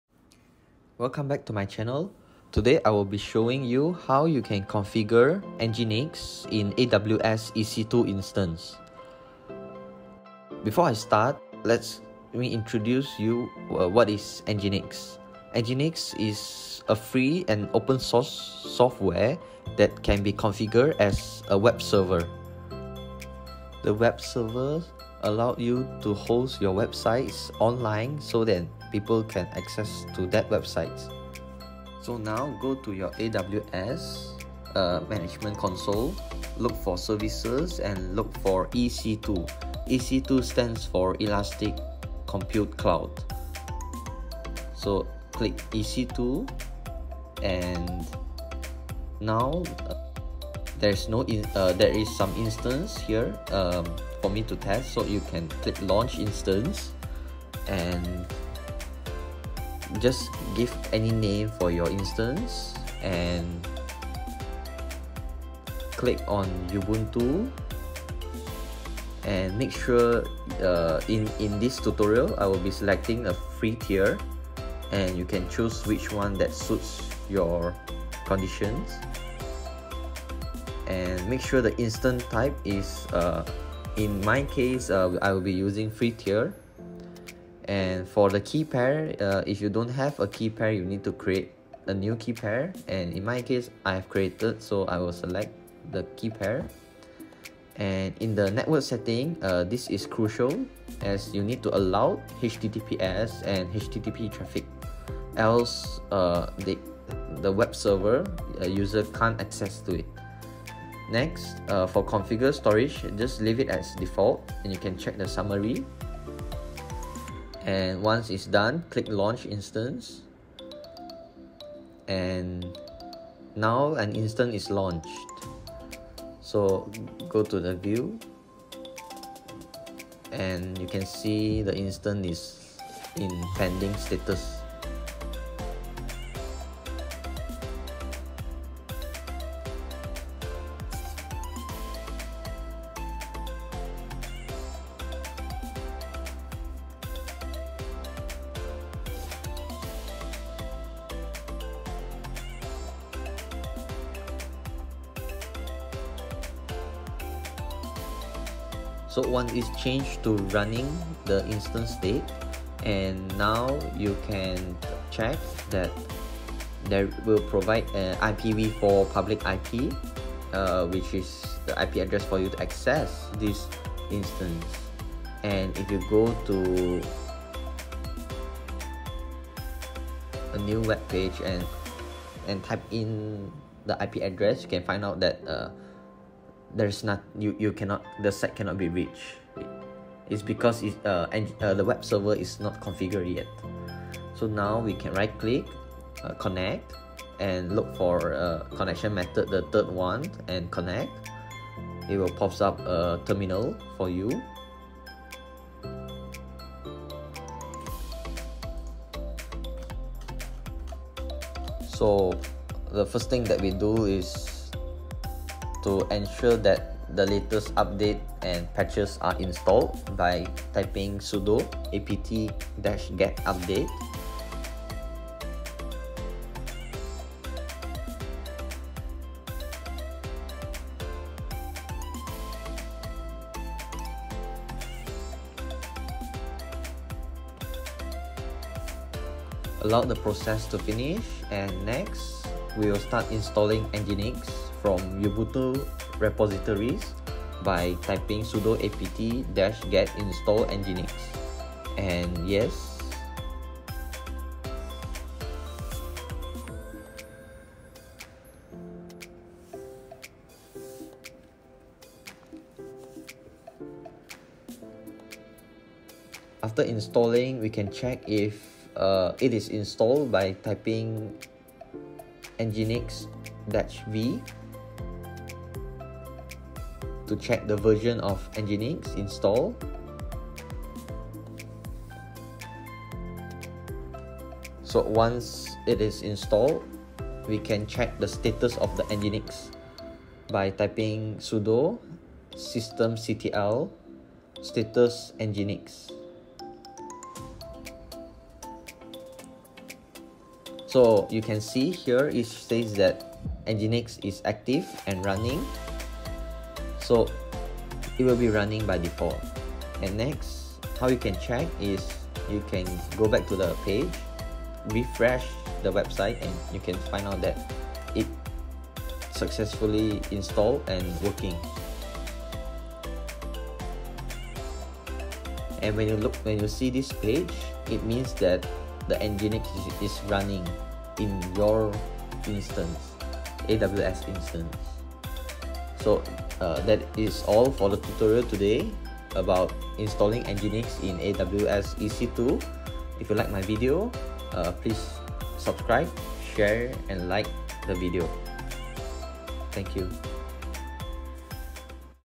How to install Nginx web server on ubuntu instance in AWS EC2 | Complete Tutorial 2022
This tutorials shows you how to configure or install Nginx as a web server on Ubuntu instance that created on Amazon Web Services(AWS) Elastic Compute Cloud(EC2).

Nginx is a free and open-source software that can be configured as a web server. Web server mainly used to host website in order for the website to be accessed online globally.